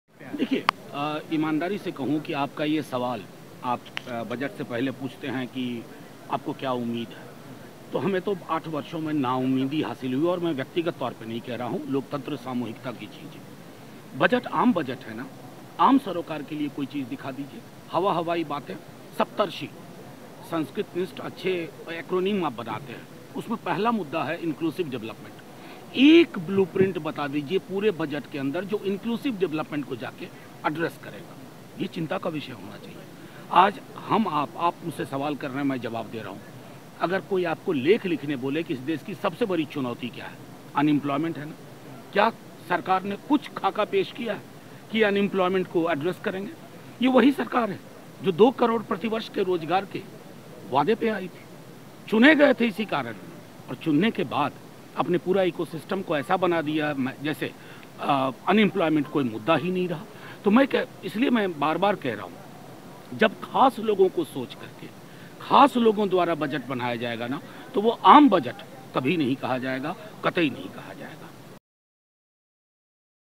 Budget 2023 | By the privileged for the privileged: Manoj Jha
Rashtriya Janata Dal leader Manoj Jha slammed the Union Budget 2023 by Finance minister Nirmala Sitharaman calling ‘Saptarishi’ farcical.

Sitharaman announced Saptarishi as one of the provisions made to set priorities for the Union Budget 2023.

“Saptarishi (which is introduced this year) is supposed to focus on inclusivity. Tell me one thing in the budget that addresses the issue of inclusivity. This is a major concern,” Jha said.

“The main problem plaguing the nation is unemployment. Has the government presented any framework to address unemployment?” RJD leader said.